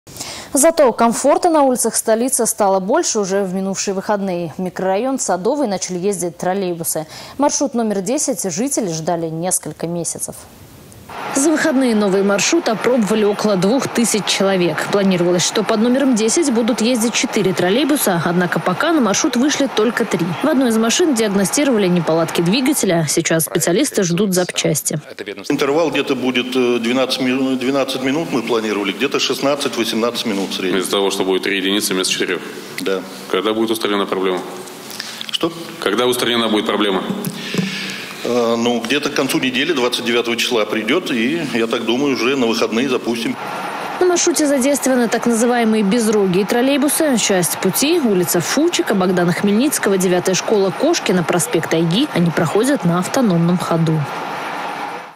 В микрорайон Садовый в Чебоксарах начали ходить троллейбусы
Маршрут №10 жители ждали несколько месяцев.